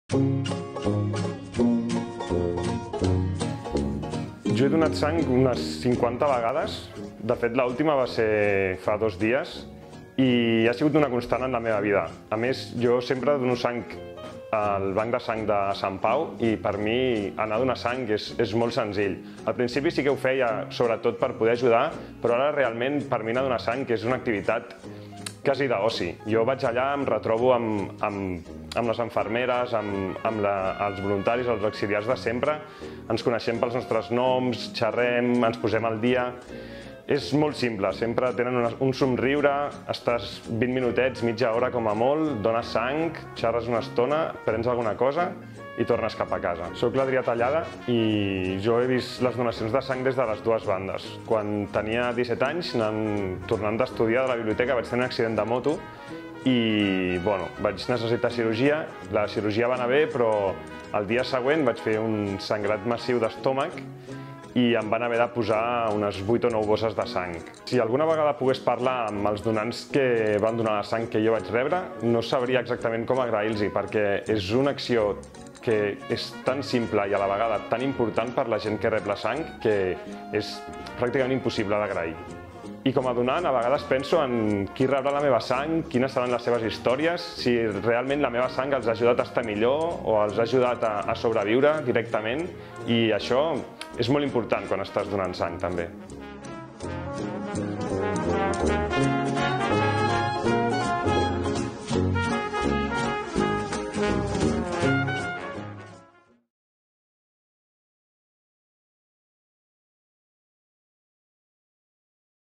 Moltes gràcies. El blog de les històries que salven una vida.
L'Adrià Tallada ha donat fins a 50 vegades. Als 17 anys va tenir un accident de moto i va necessitar fins 9 bosses de sang